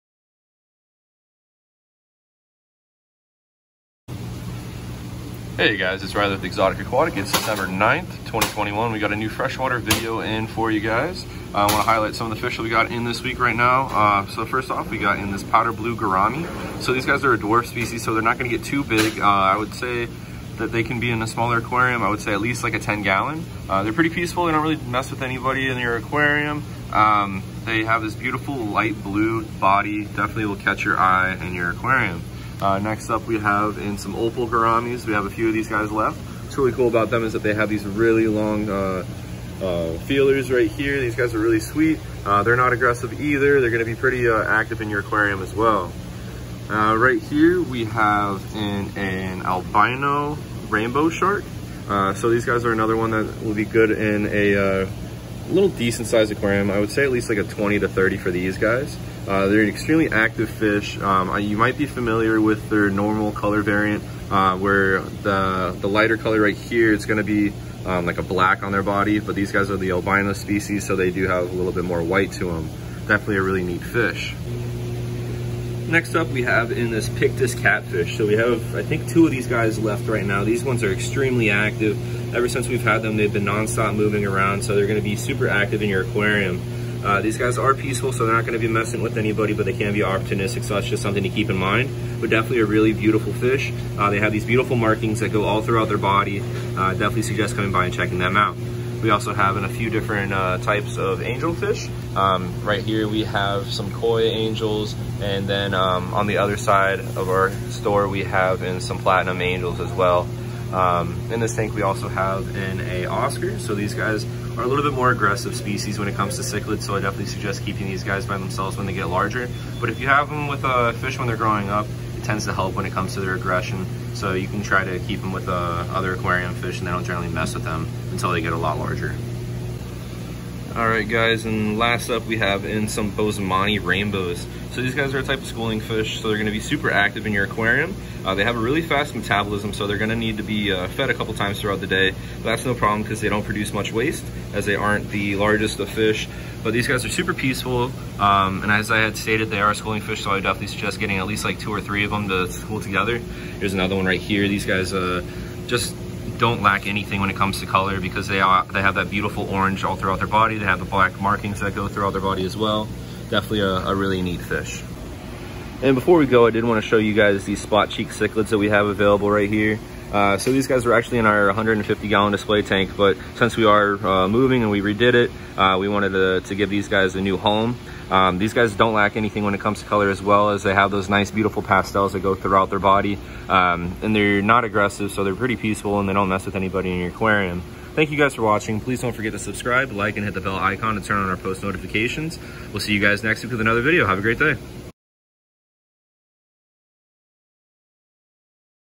Freshwater Arrivals 12:9:21
Riley shows us some new freshwater fish that came in this week. Come by and check them out!